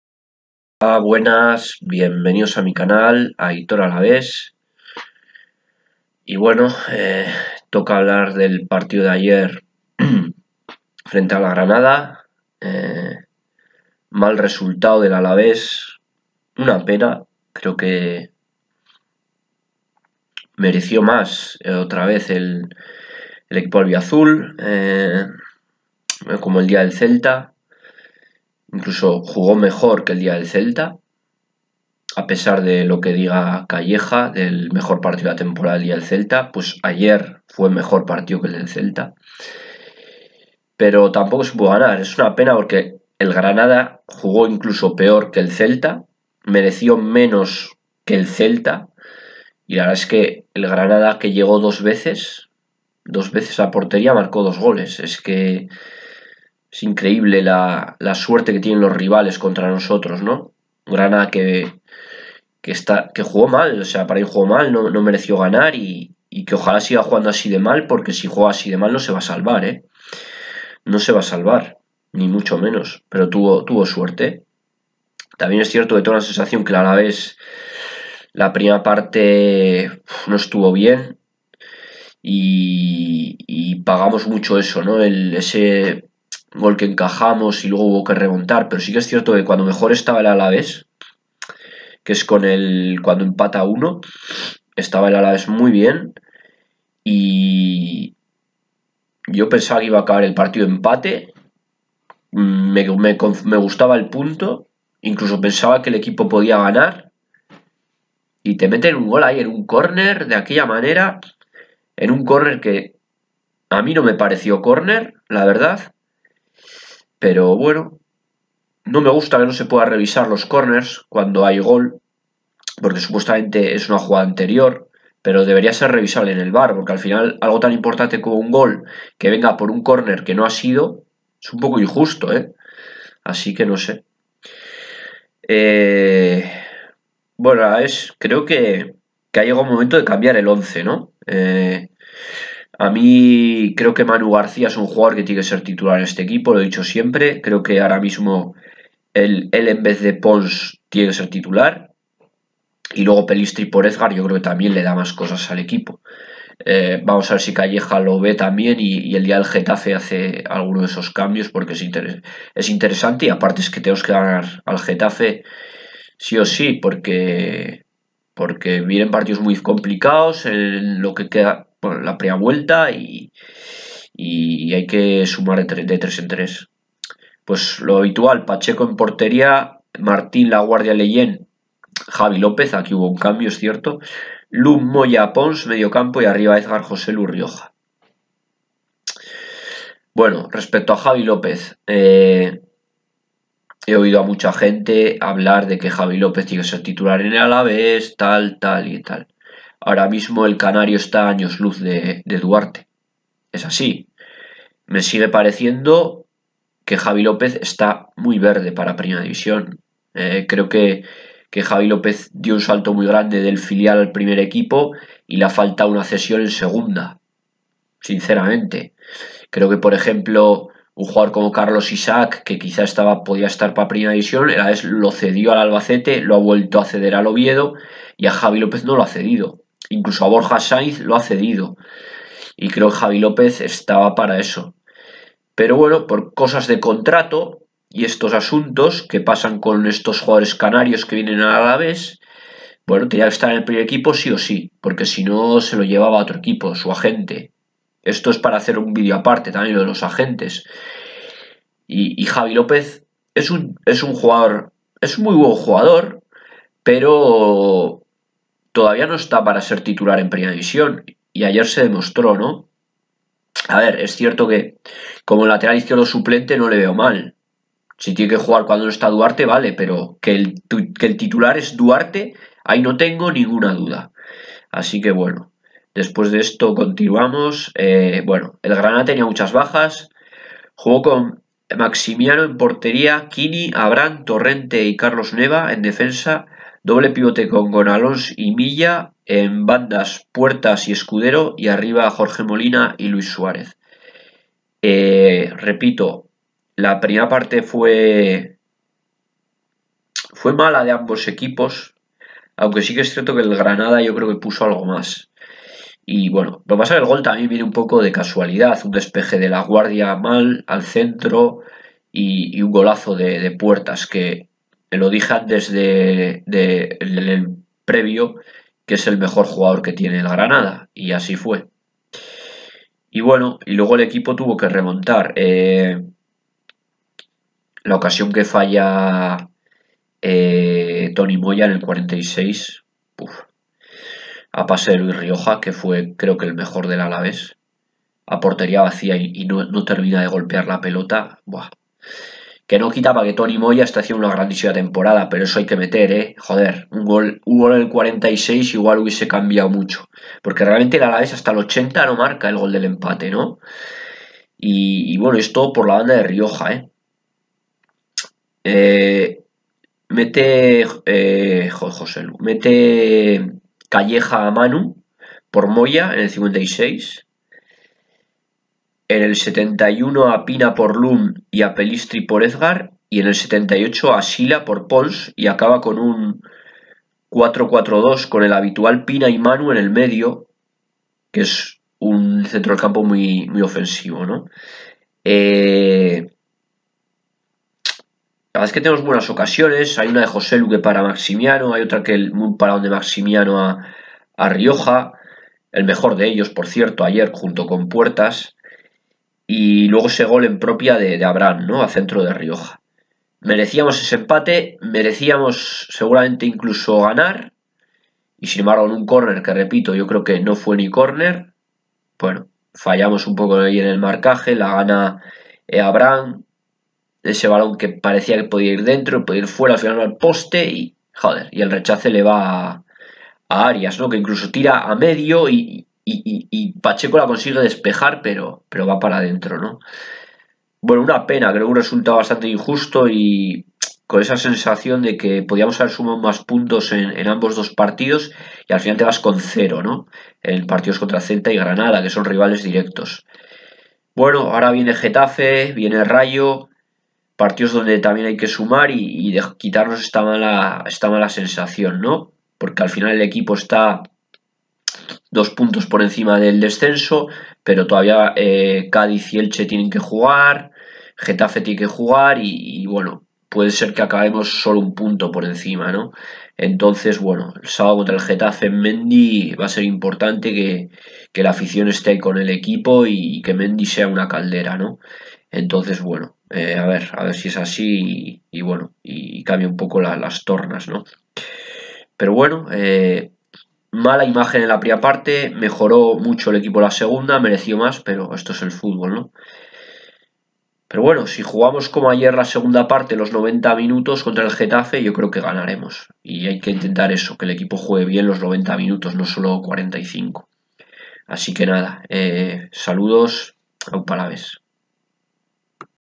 Granada 2-1 Alavés
El Alavés perdió 2-1 en el Nuevo Los Cármenes en un partido en el que mereció mejor resultado